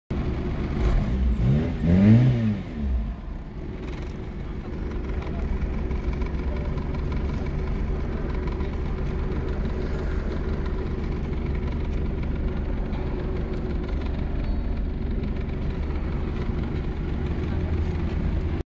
Testing Proton Saga LMST 2006
This car is a Used unit & i've testing this car & it quite good, i really love old cars!